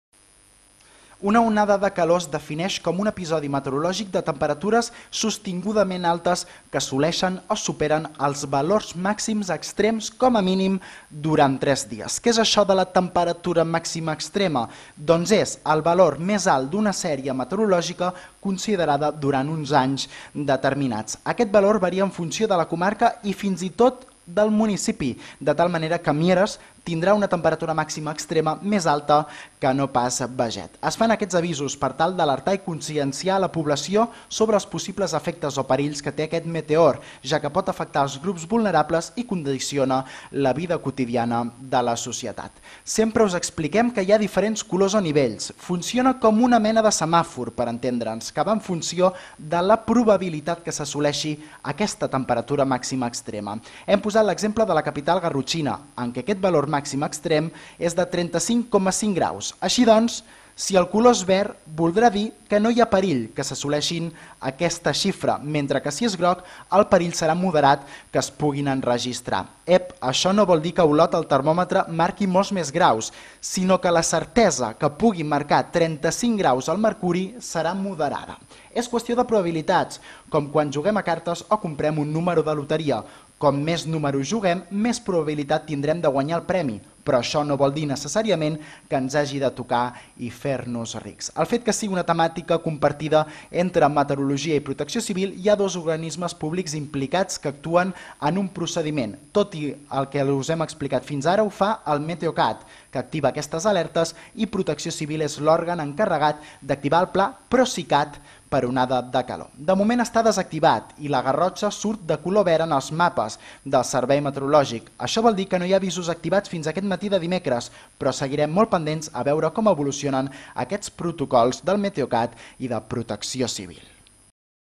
Avisos d’onada de calor durant aquest període d’altes temperatures persistents
Una onada de calor és defineix com un episodi meteorològic de temperatures sostingudament altes que assoleixin o superin els valors màxims extrems com a mínim durant tres dies. El valor ºC màx. Extrema és la temperatura més alta d’una sèrie meteorològica considerada durant uns anys determinats. El valor varia en funció de la comarca i fins i tot del municipi, de tal manera que Mieres tindrà una temperatura màxima extrema més alta que no pas Beget, per exemple. Es fan aquests avisos per tal d’alertar i conscienciar la població sobre els possibles efectes o perills que té aquest meteor, ja que pot afectar els grups vulnerables i condiciona la vida quotidiana de la societat.